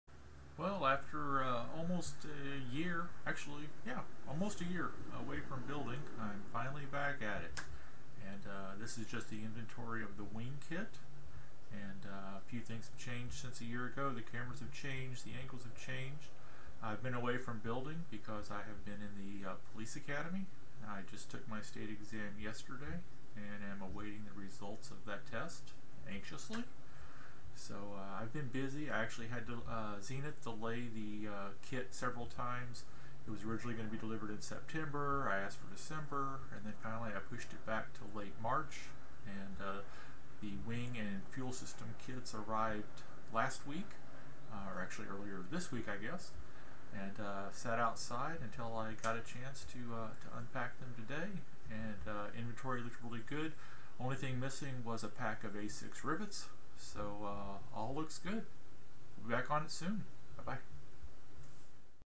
Episode 23 - Wing Kit Inventory
This is just taking inventory of the wing and fuel system kits. The spars are not shown, but are overhead, as are the fuel tanks. This is the first video in almost a year because I have been in the police academy and asked Zenith to delay this kit a few times. It finally came in this week and I took about an hour to today to unpack and inventory everything. Looks good, despite Estes being real hard on the shipping crate!